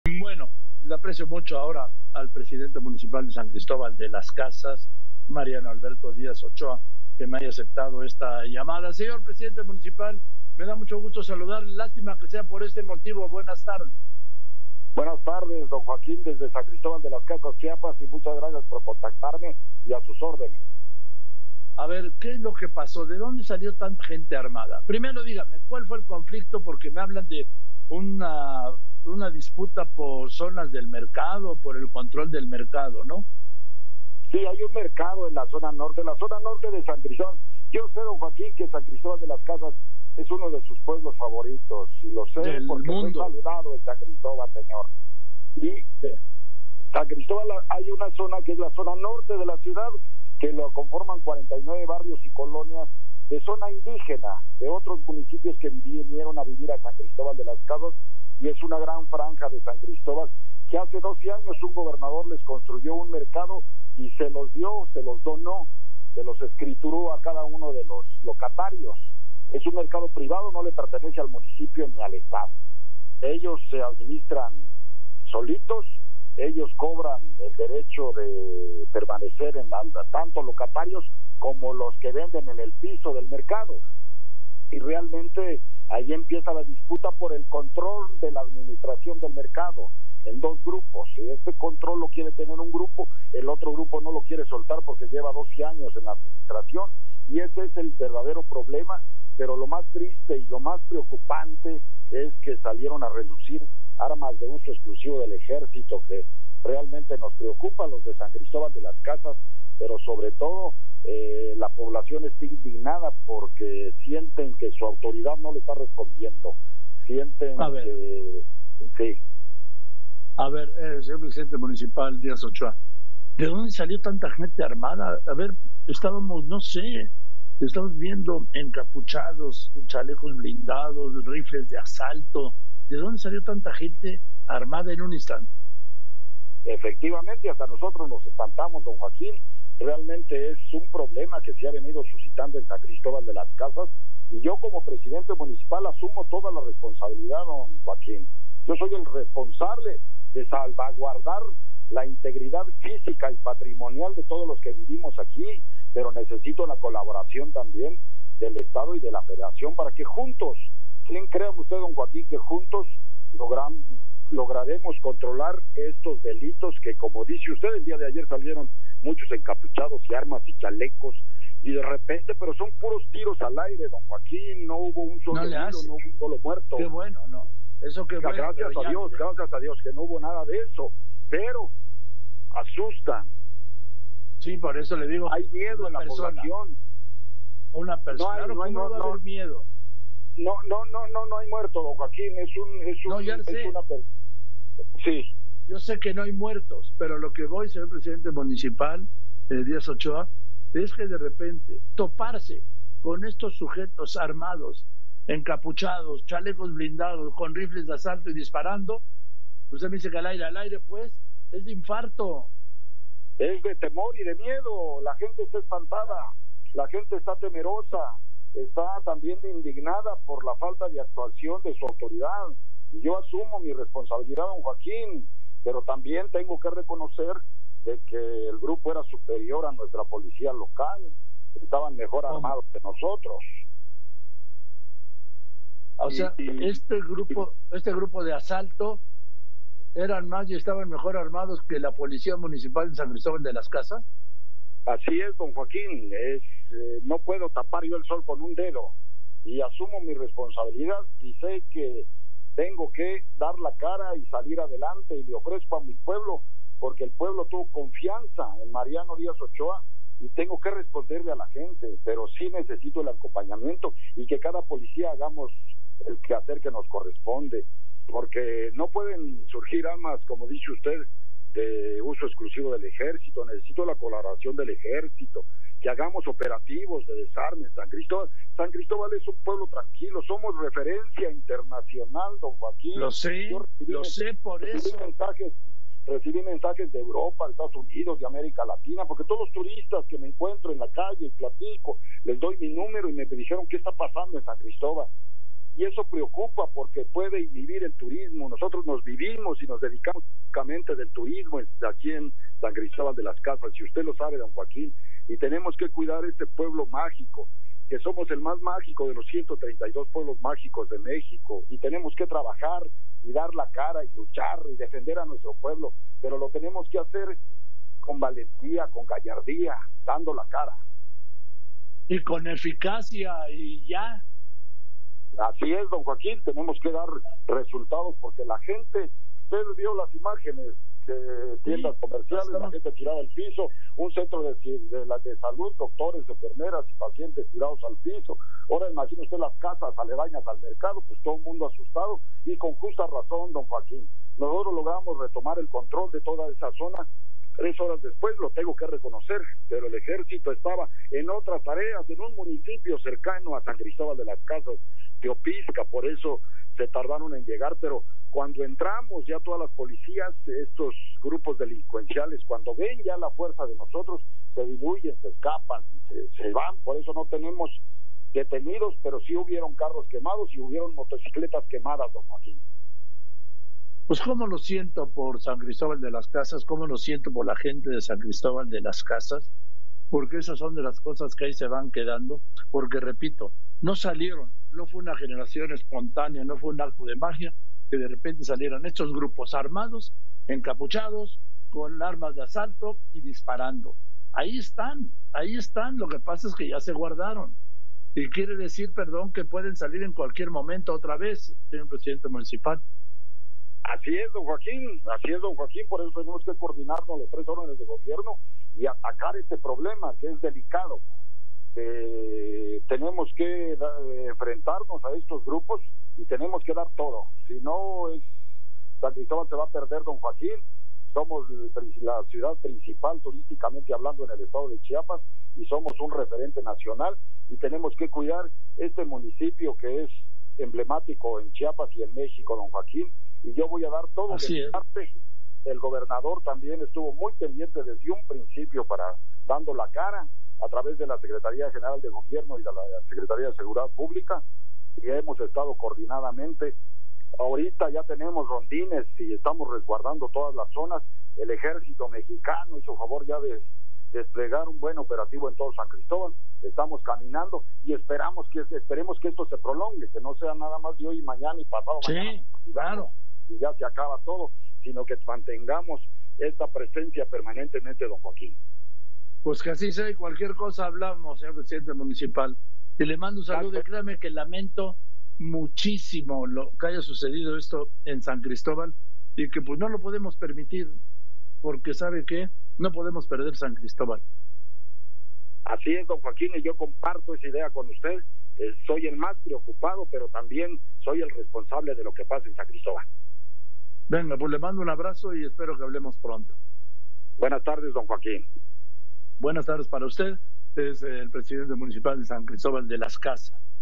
No hay detenidos por enfrentamiento en San Cristóbal, Chiapas
Mariano Alberto Díaz Ochoa, presidente Municipal de San Cristóbal de las Casas, Chiapas, confirma que no hay detenidos por el enfrentamiento de este martes en el mercado